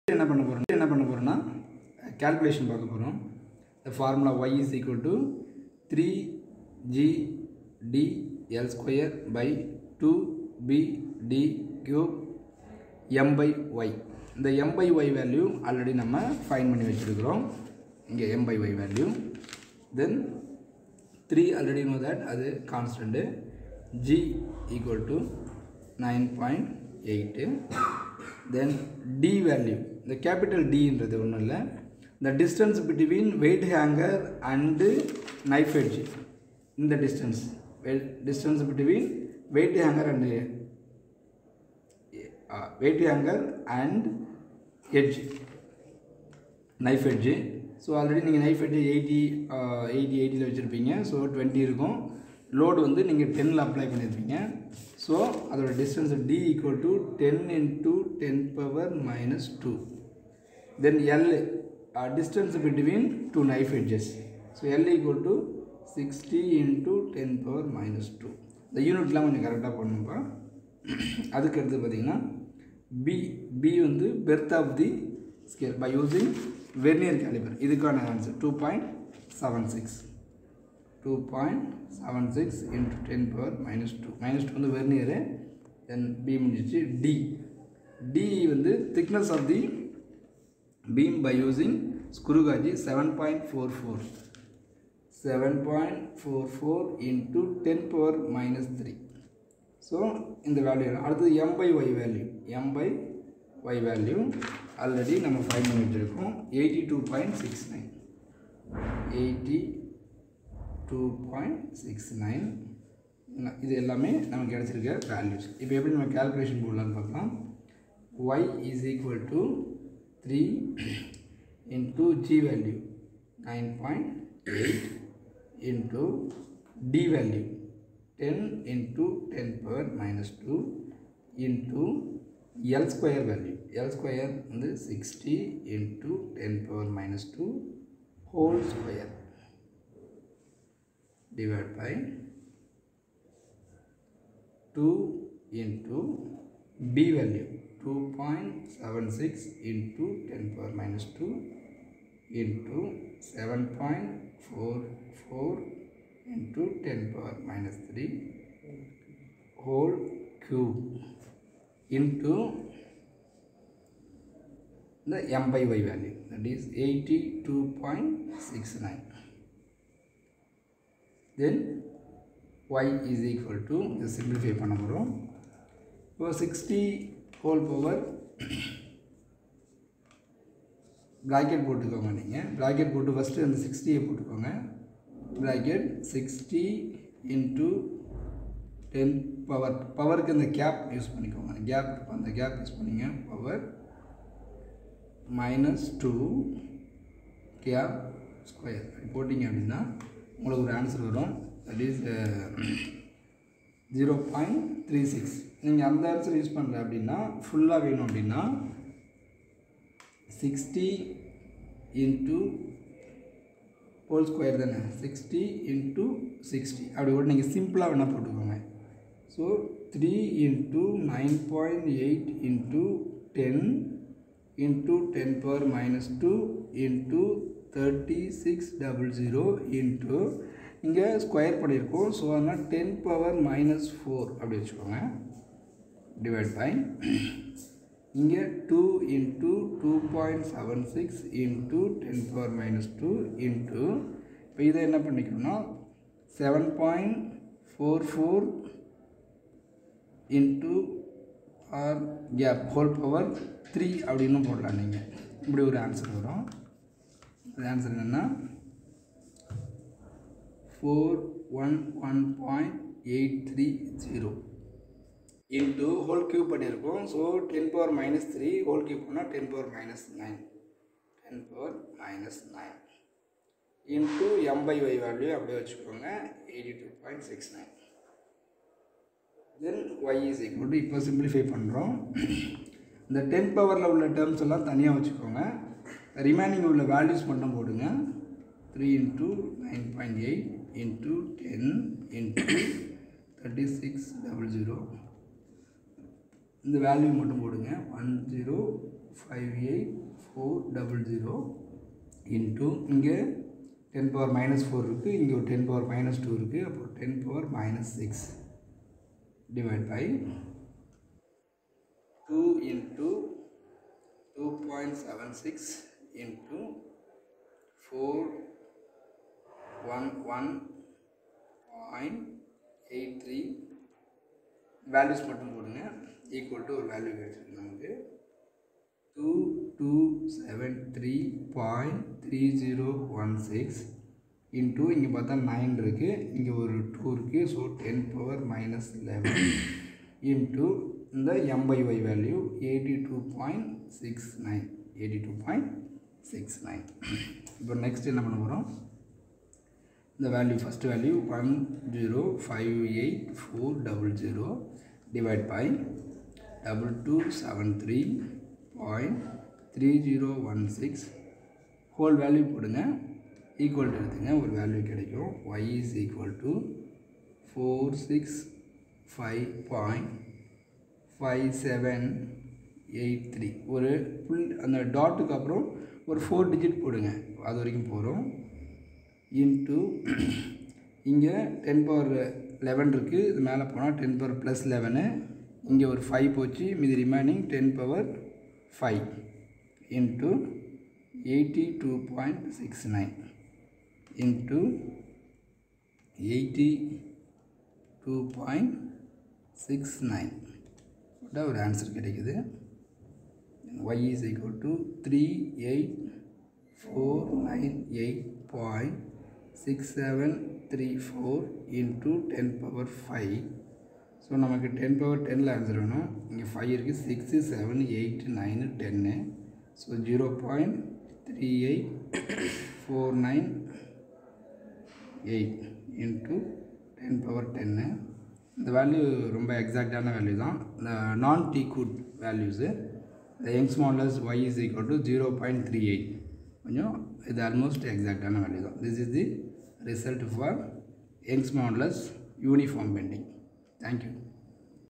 Young's modulus Uniform bending experiment part 2 - BS3171
How to calculate Young's modulus of a Wooden scale